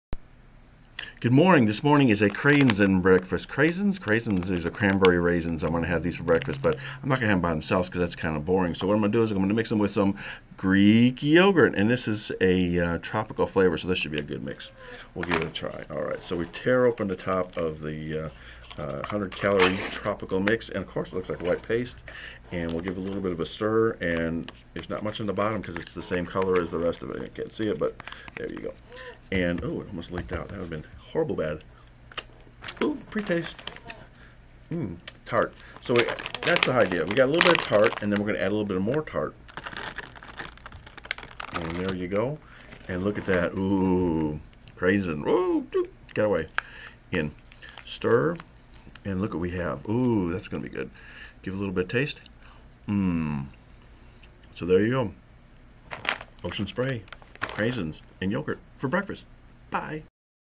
Is it pronounced Cran-sin or Cray-sin?
However you call them, we mix some in a tart tropical fruit yogurt.